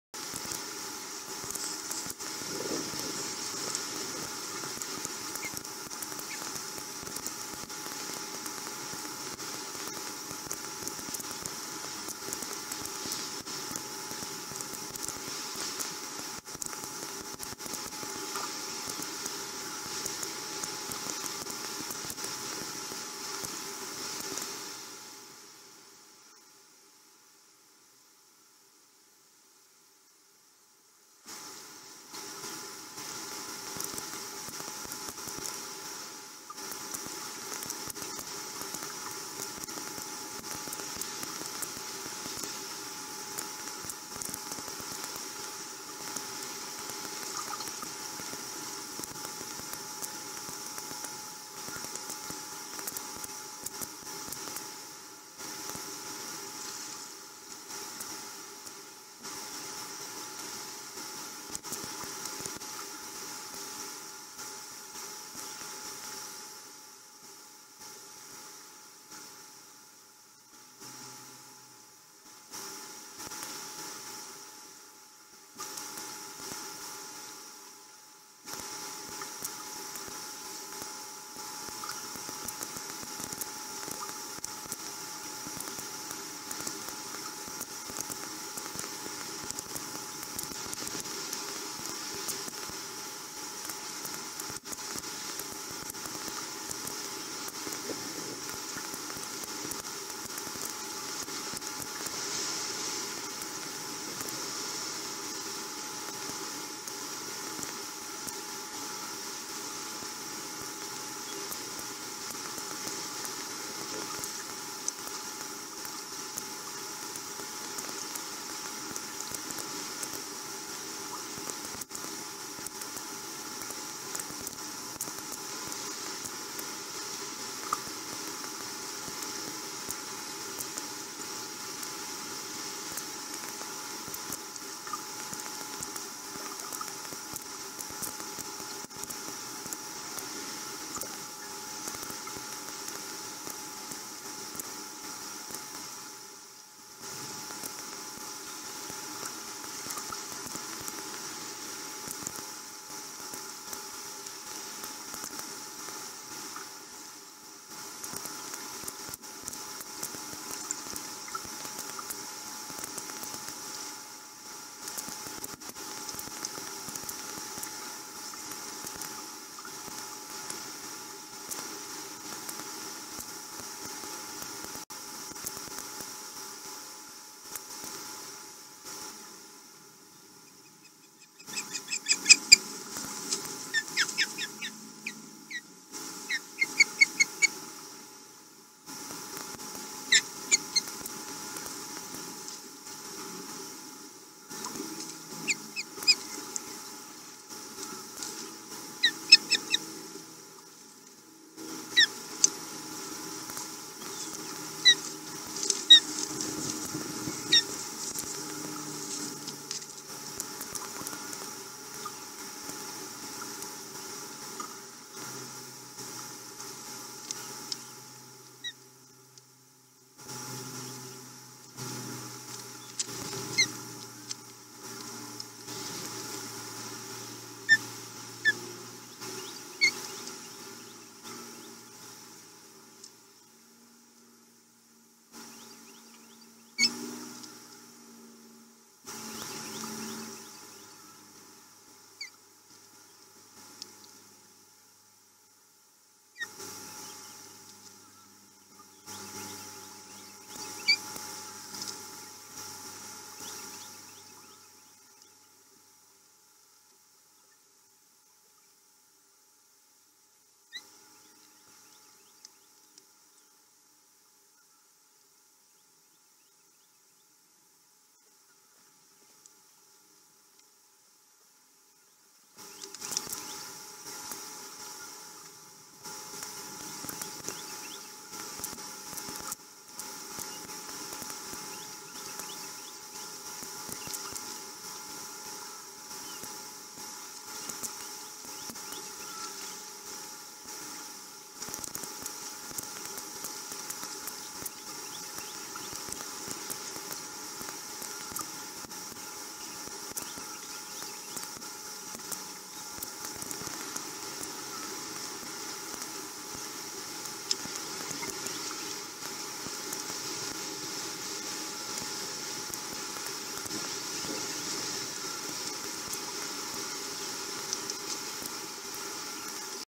Chesap06-07-2020/Tom with fishtail, Aud finish it
Tomfly in with a fishtail,waits still and left.Audrey comes home and found the tail and finished the fishtail.